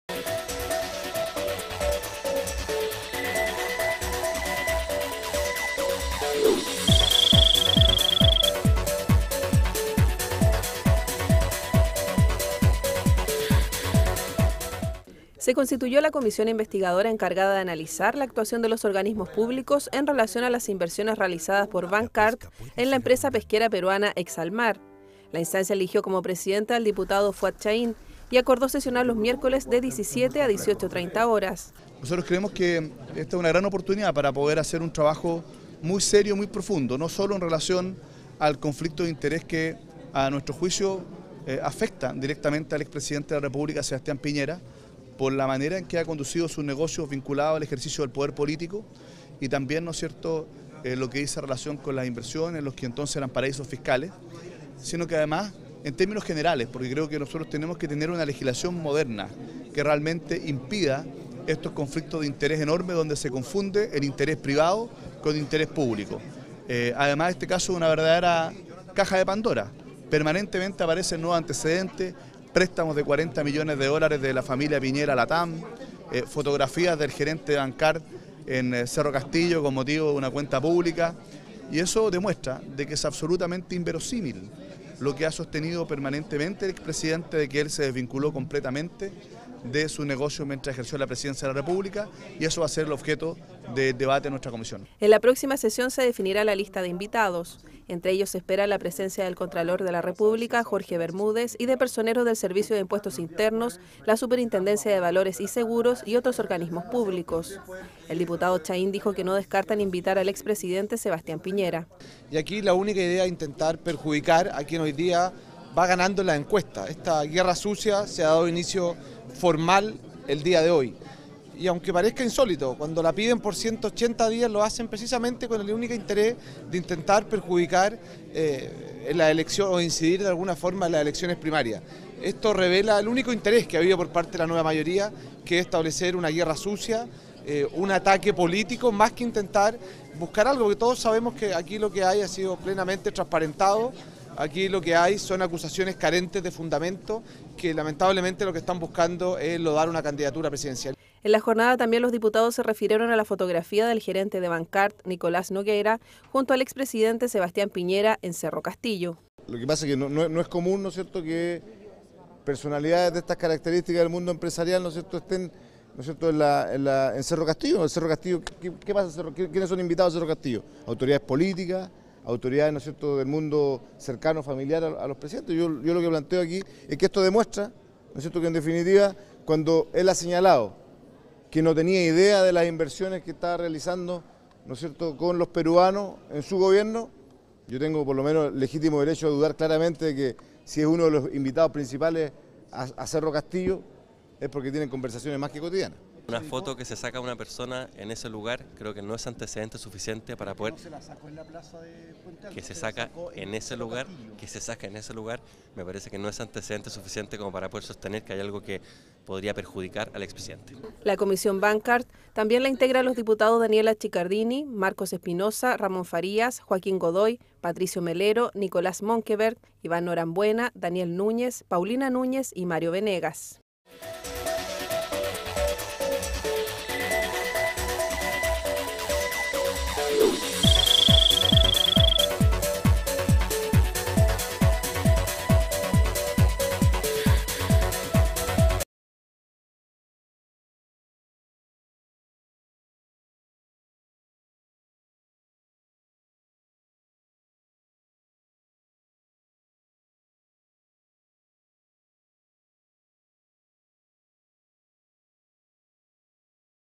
CONSTITUYEN COMISIÓN INVESTIGADORA SOBRE INVERSIONES DE BANCARD
Analizará actuación de organismos públicos respecto a inversiones de Bancard en empresa peruana Exalmar.